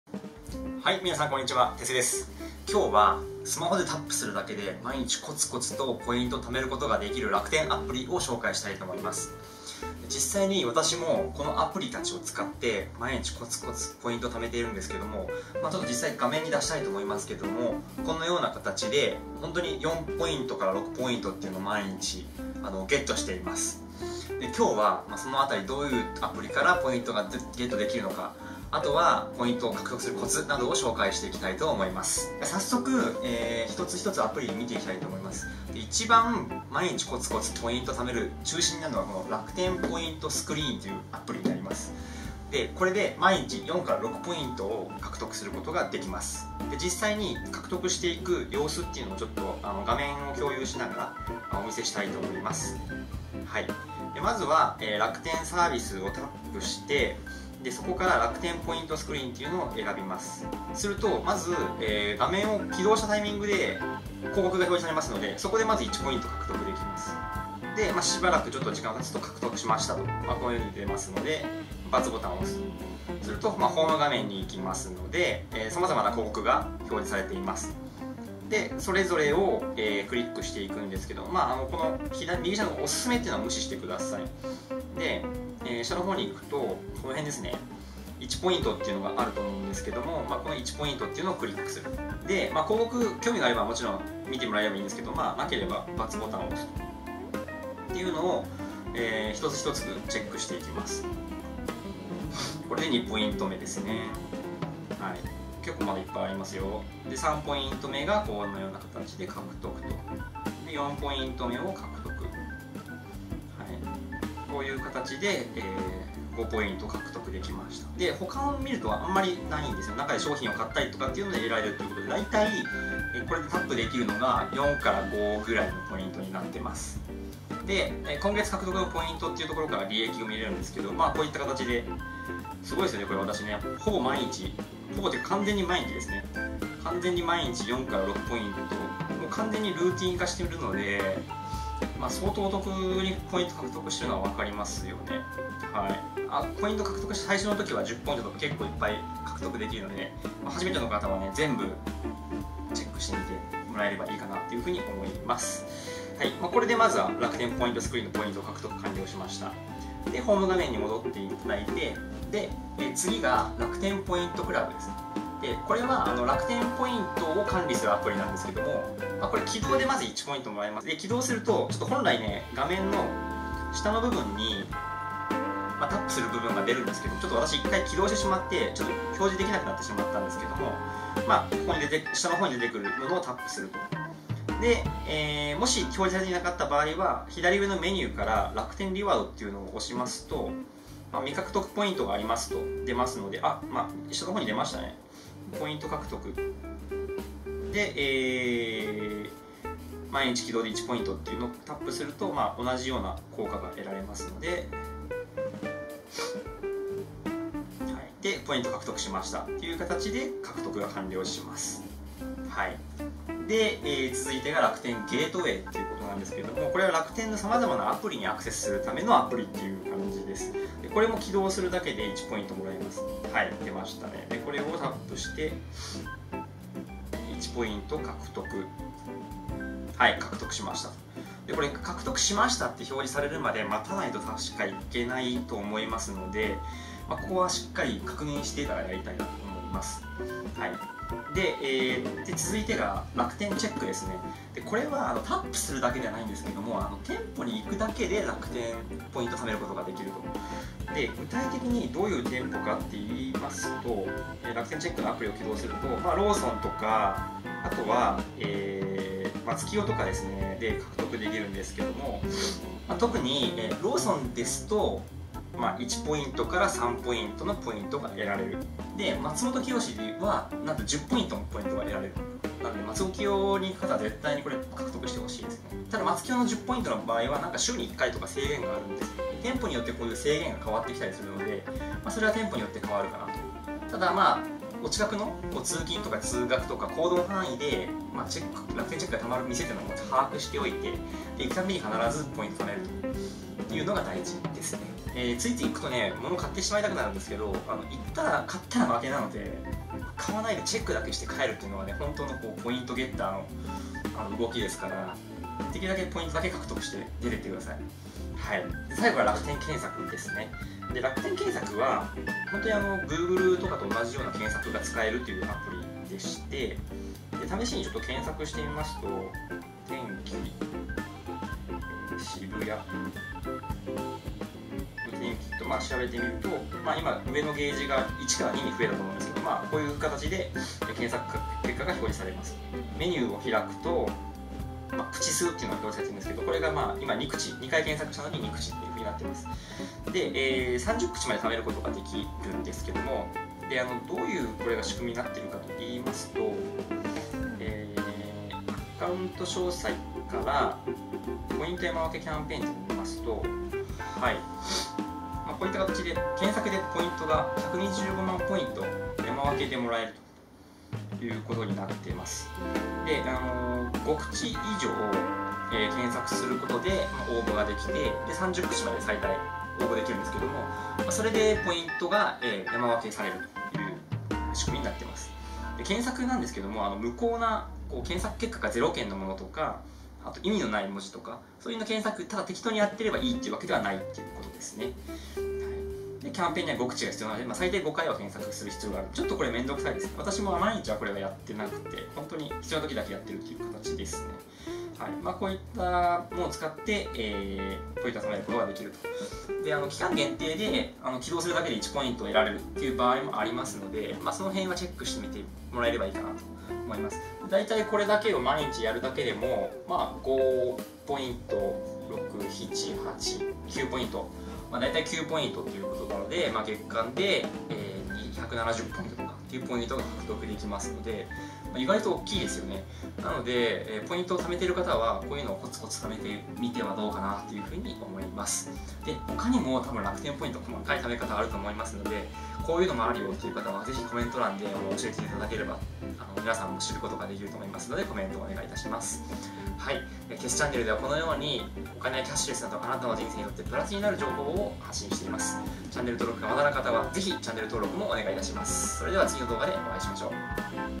スマホでタップするだけ！楽天ポイントを毎日コツコツ貯めることができるアプリ紹介
知っているか知らないかで変わる。スキマ時間を活用して楽天ポイントを貯める方法を紹介しています。

この方法をやることで、毎日8ポイント前後の楽天ポイントを無料で獲得することができます。

補足：楽天ゲートウェイのポイント付与は無くなったようです。2021年2月2日追記